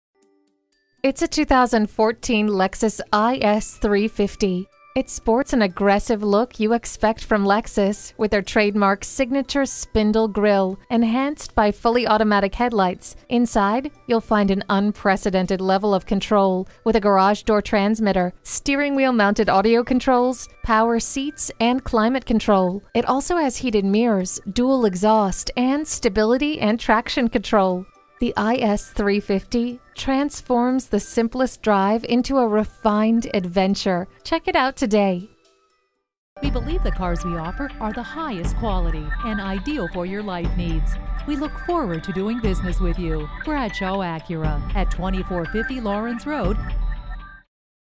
Used 2014 Lexus IS Greenville SC Easley, SC AP5203 - SOLD
Year: 2014
Make: Lexus
Model: IS
Trim: 350
Engine: 3.5L V6 DOHC VVT-i 24V
Transmission: 6-Speed Automatic Electronic with ECT-i
Color: Ultra White
Interior: Saddle
Mileage: 29614
Stock #: AP5203
VIN: JTHCE1D2XE5005263

Address:
2450 Laurens Rd